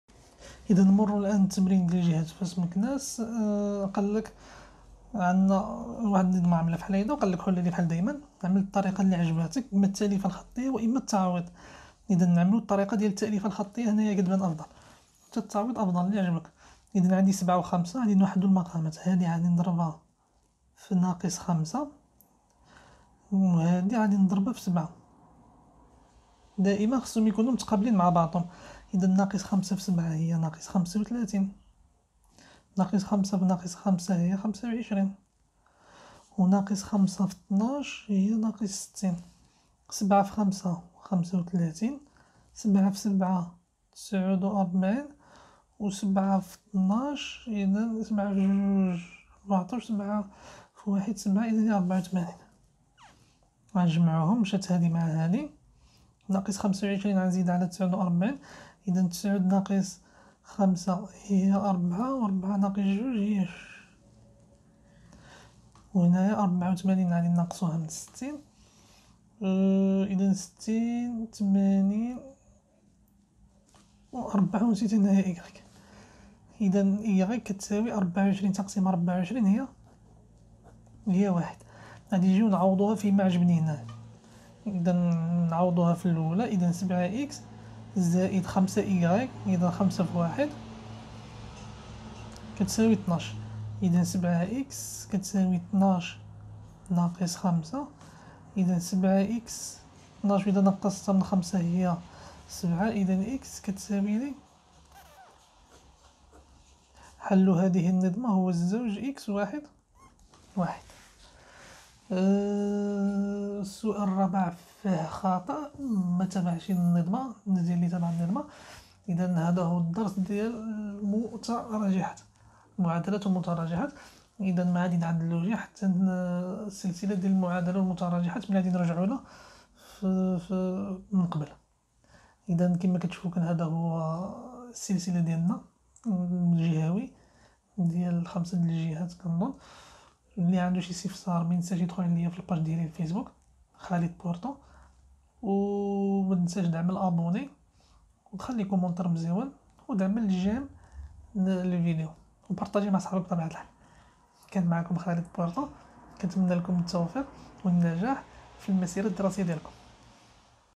النظمات والمسائل - تمارين من إمتحانات 2018 الجهوية (جزء 5)
تمارين في النظمات والمسائل من الإمتحانات الجهوية 2018
الثالثة إعدادي الخيار الوطني والدولي
للاستفسار: